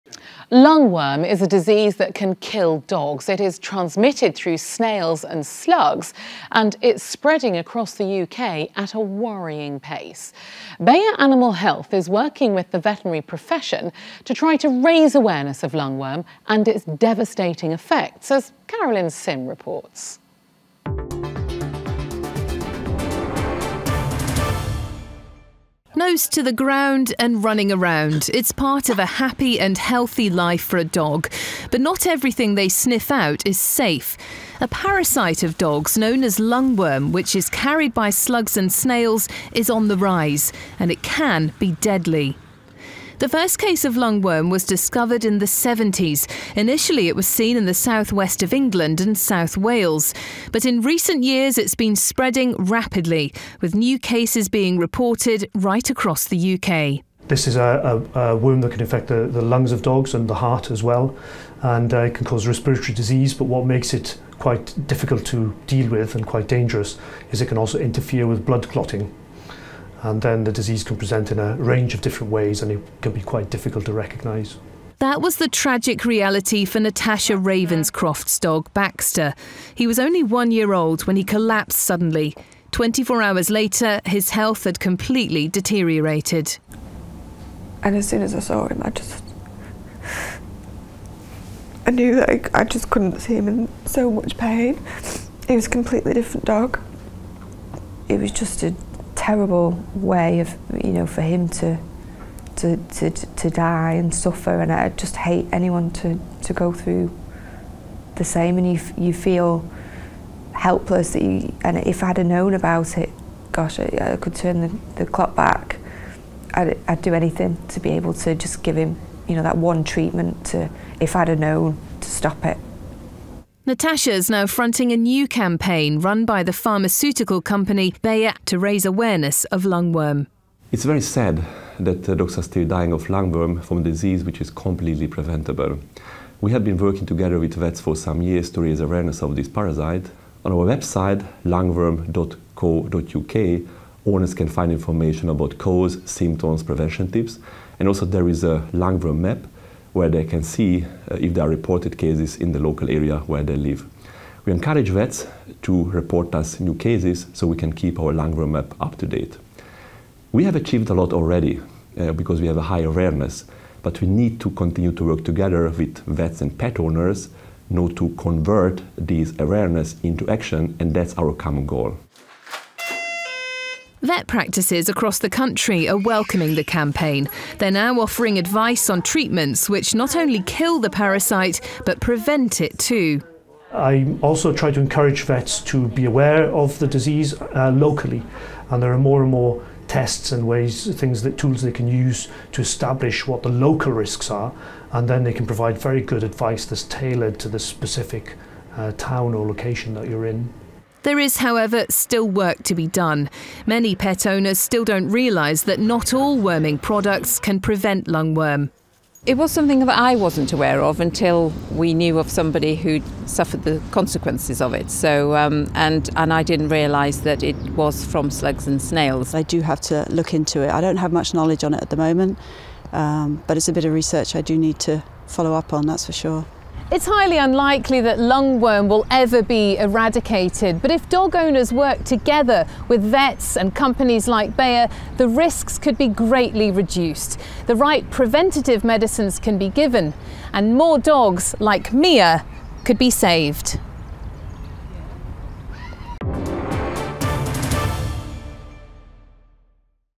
How can we prevent the spread of lungworm? - Bayer Animal Health
Lungworm is a disease that can kill dogs. It is transmitted through snails and slugs and it's spreading across the UK at a worrying pace. Bayer Animal Health is working with the veterinary profession to try to raise awareness of lungworm and its devastating effects.

to find out more about the dangers of lungworm